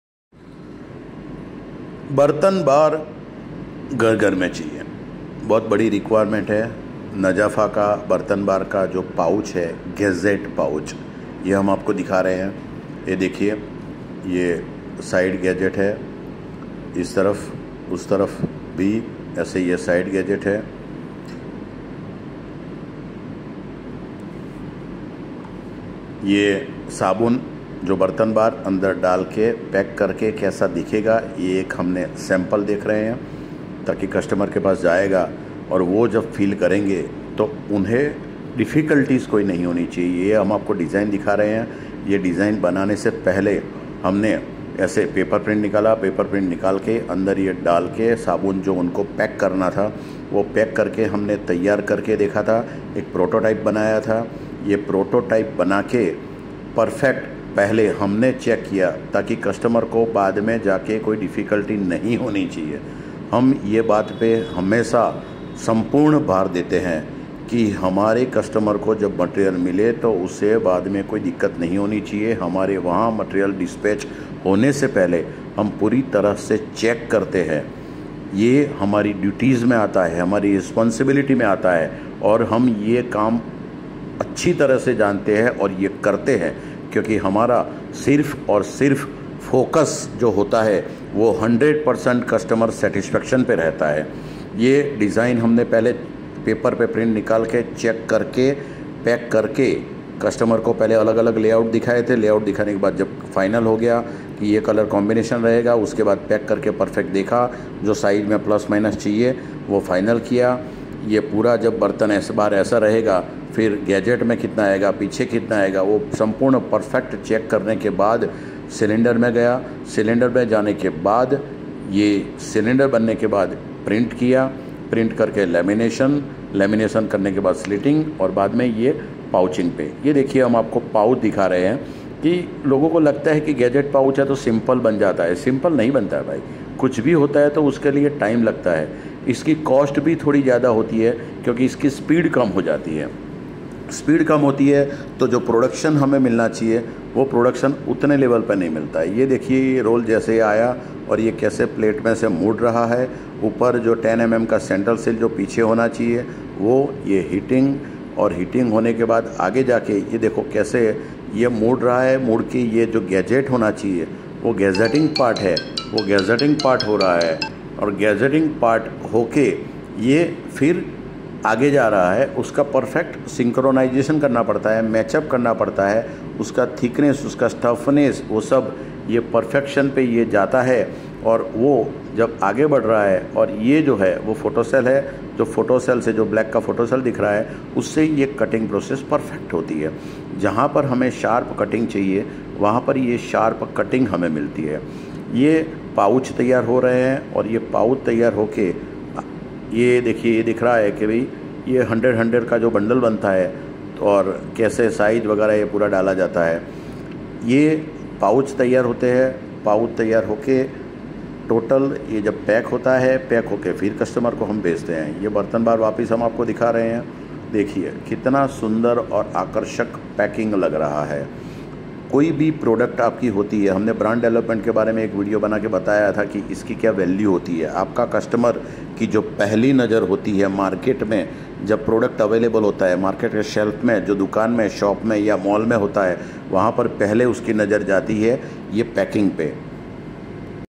Bartan Bar Printed Laminated Packaging Pouch Making Process | Detergent Powder | Soap Pouch Rolls
Bartan Bar Gusset Pouch | Soap Pouch | Detergent Printed Laminated Pouch Making Process at Emkay Flexipack.
30 Years 100 % Customer satisfaction track record.